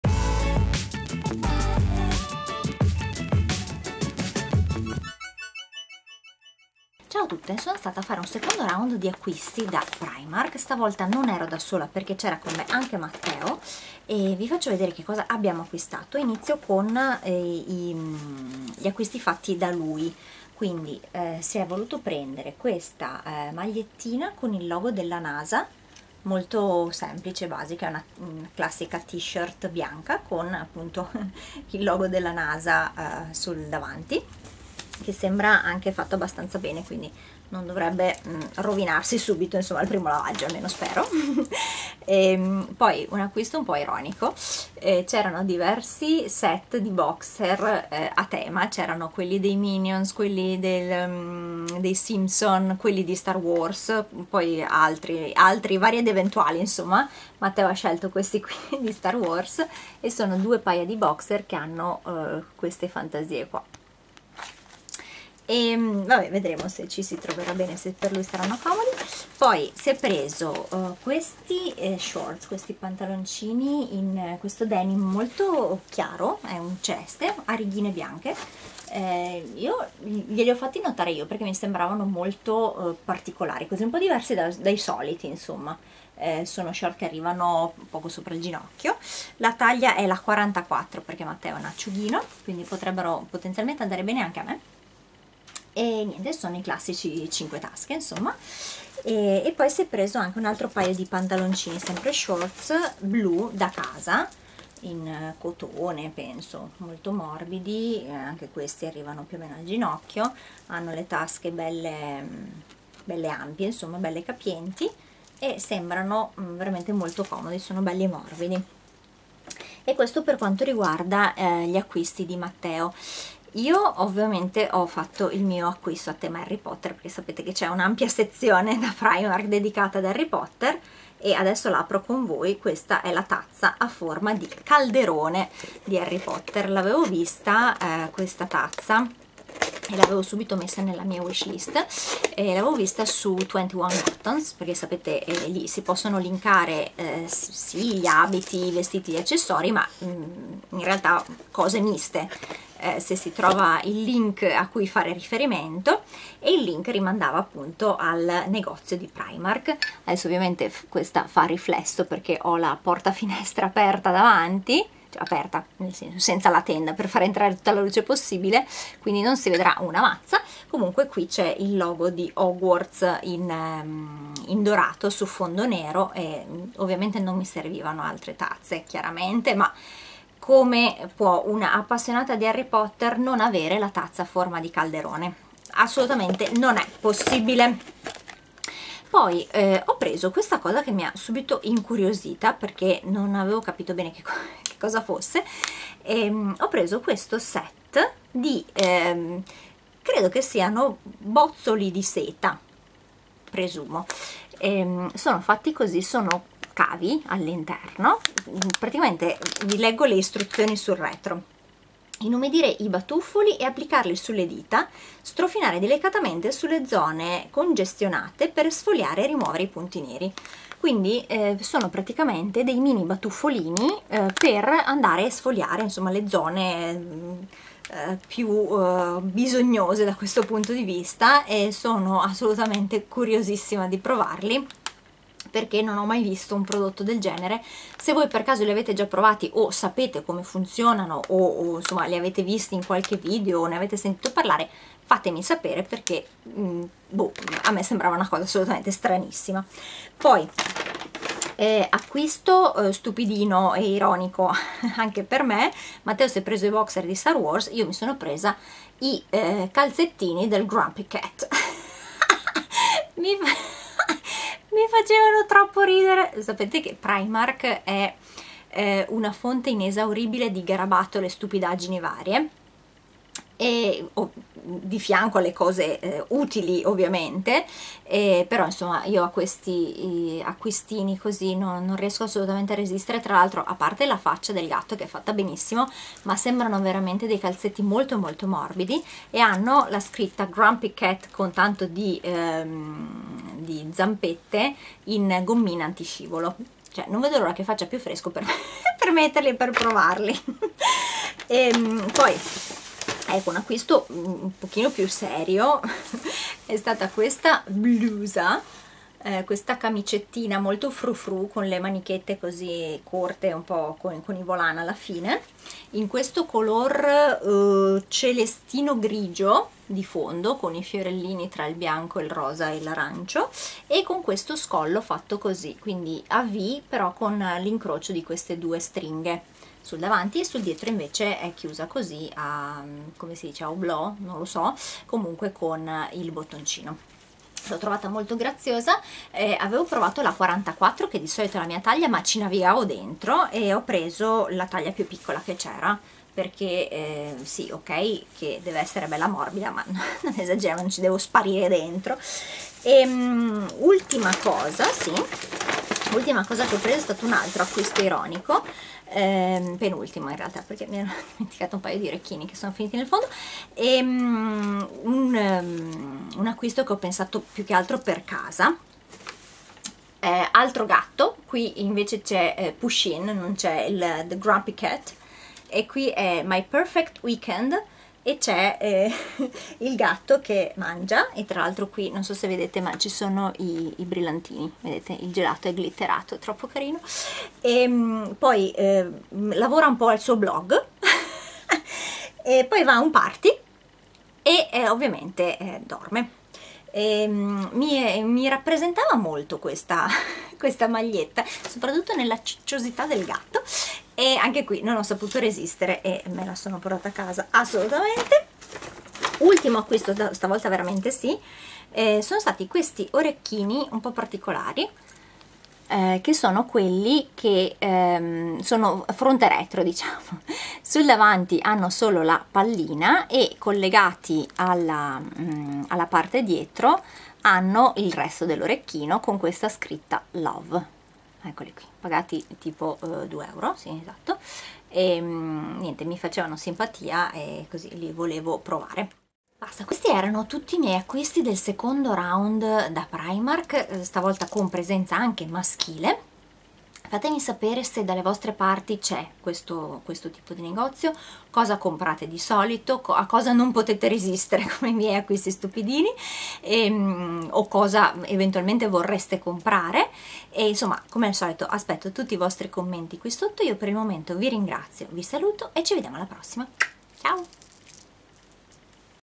Haul || Secondo round da Primark
Vi scrivo i prezzi dei prodotti nell'ordine in cui ve li ho mostrati nel video:
- t-shirt NASA: € 7,00
- set 2 boxer Star Wars: € 9,00
- short uomo in denim a righe: € 8,00
- short blu: € 10,00
- tazza calderone Harry Potter: € 6,00
- set 12 bozzoli di seta: € 2,00
- calzetti Grumpy Cat: € 3,00
- camicetta a fiori: € 7,00
- t-shirt Pusheen Weekend: € 8,00
- orecchini Love: € 2,00